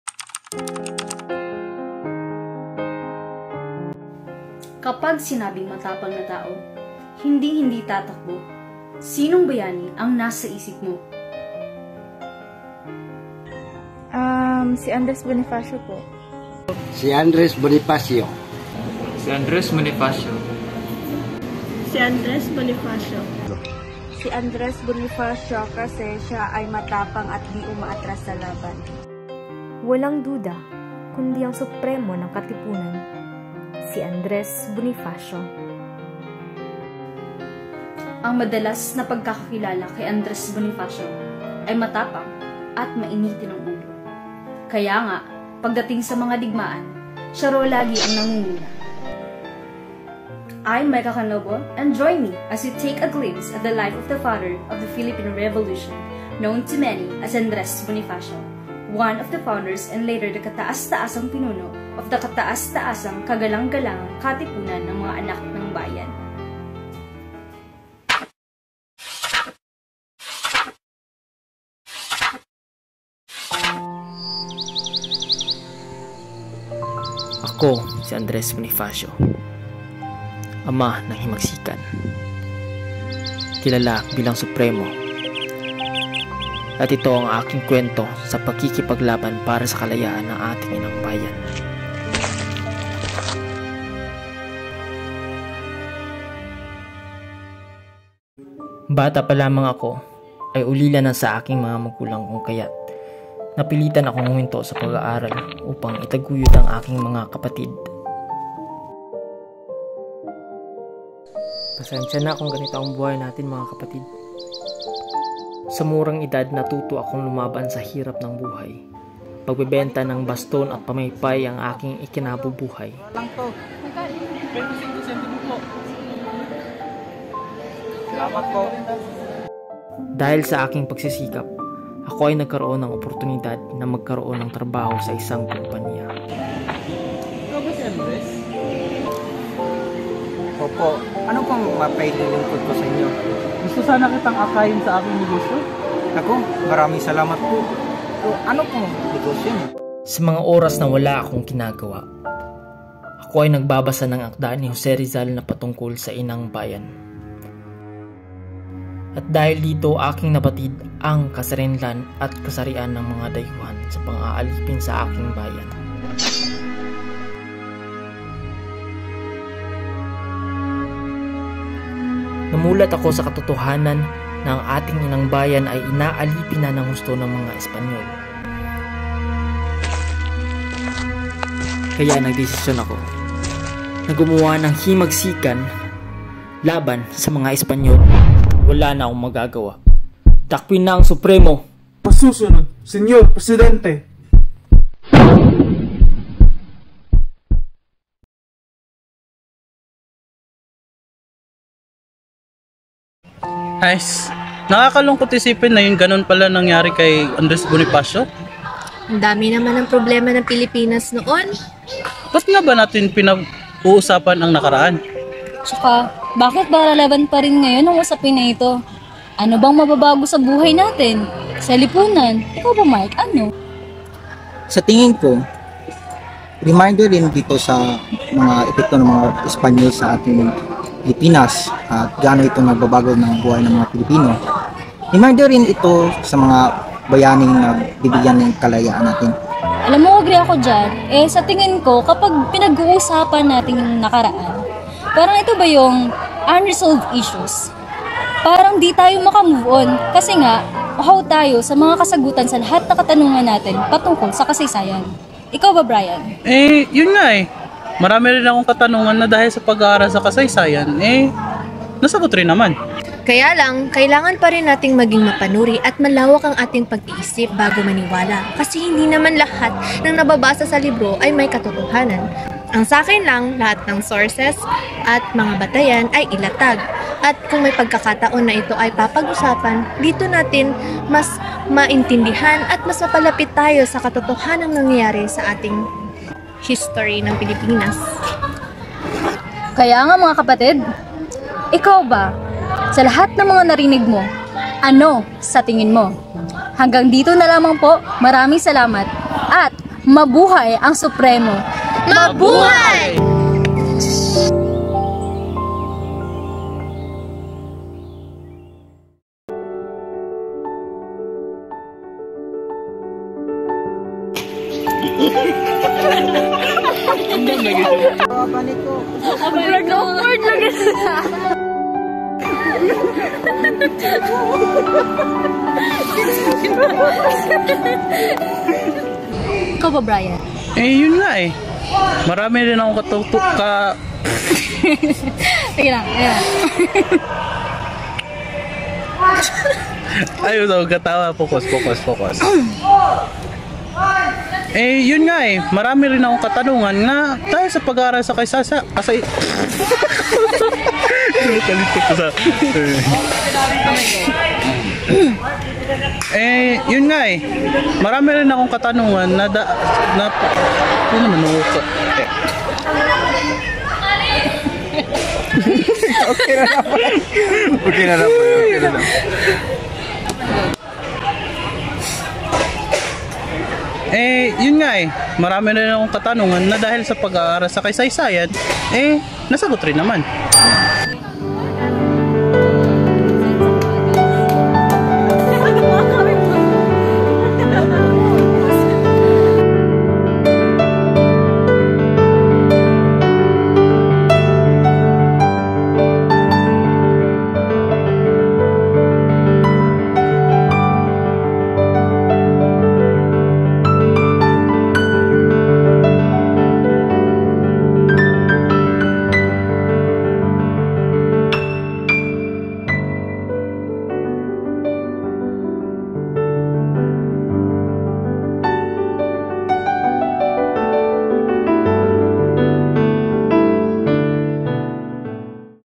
A Short Film on ANDRES BONIFACIO y DE CASTRO "The Father of the Philippine Revolution" (NMV#21)
This is part of the final requirement for my GE-8 College of Engineering Students.

Filipino Freemason and revolutionary Andrés Bonifacio y de Castro. He is regarded as one of the national heroes of the Philippines and is frequently referred to as "The Father of the Philippine Revolution."

Readings in Philipine History: ANDRES BONIFACIO